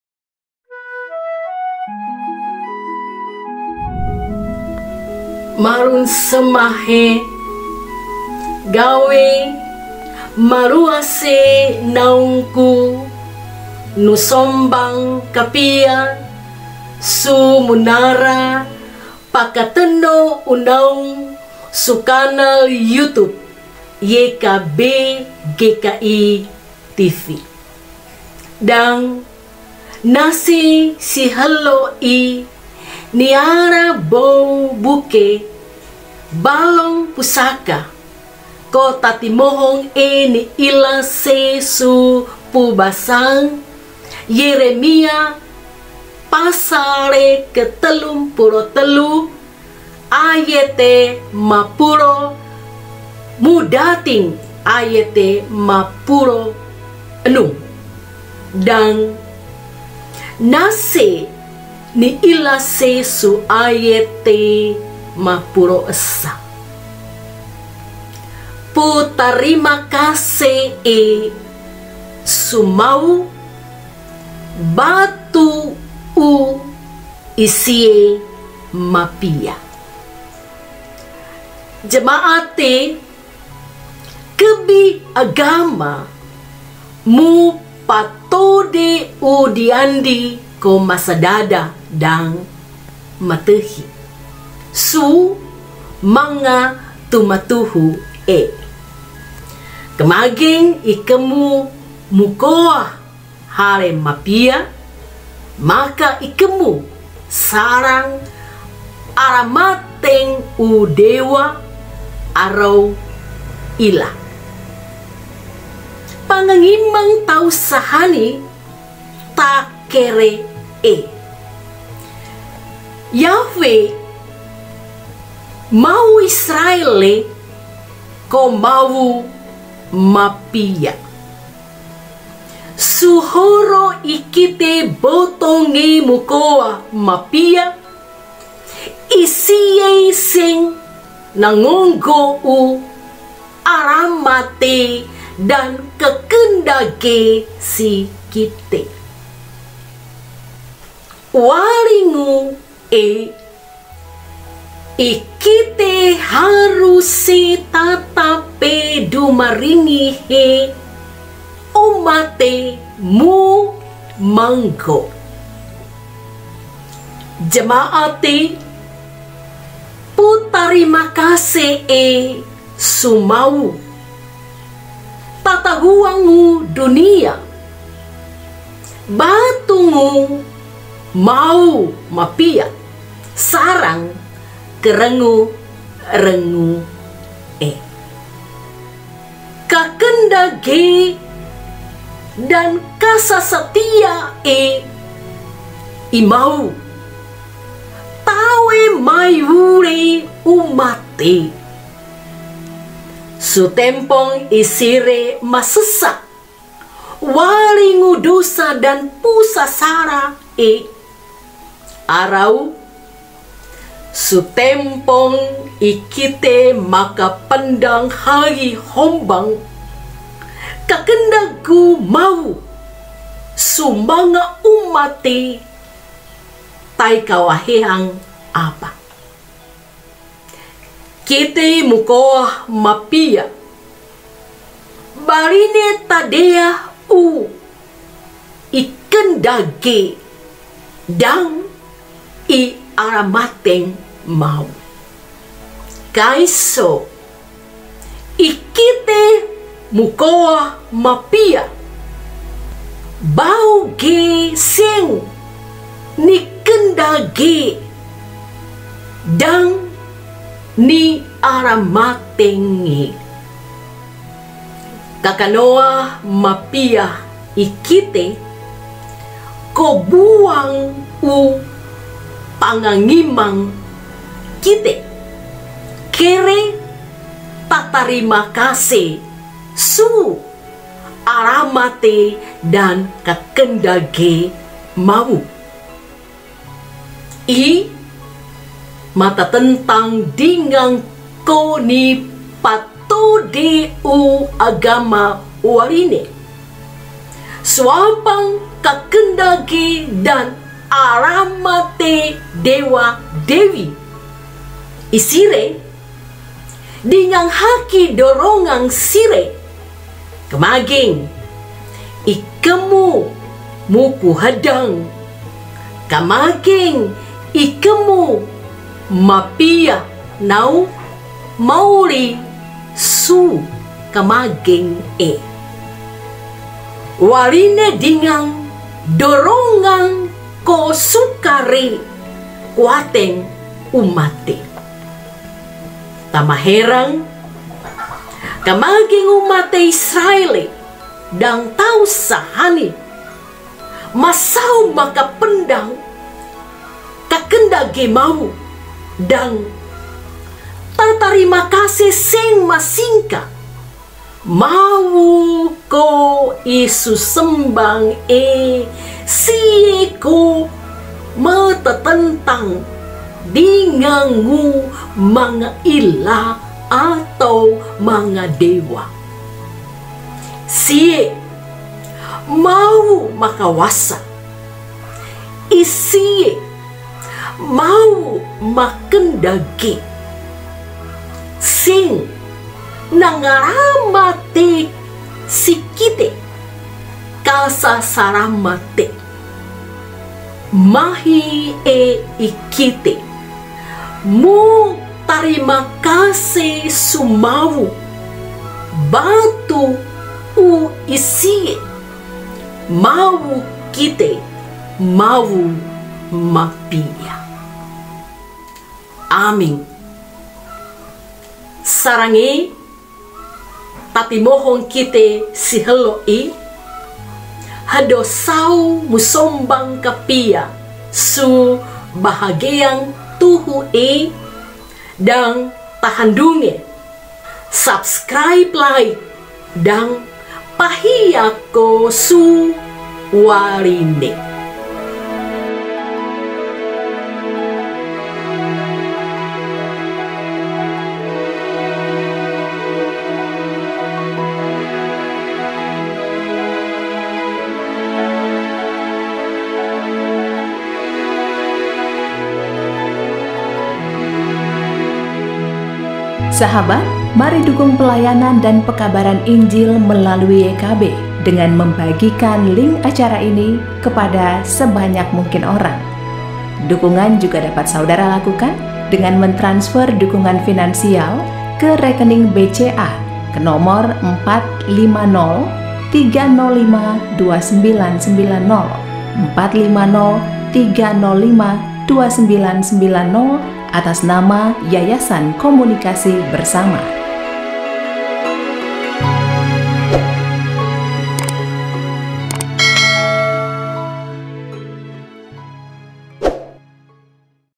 PAKATENO U NAUNG 008 - PUTARIMAKASE E SU MAWU BATU U I SIE MAPIA
Renungan firman Tuhan dalam bahasa Siau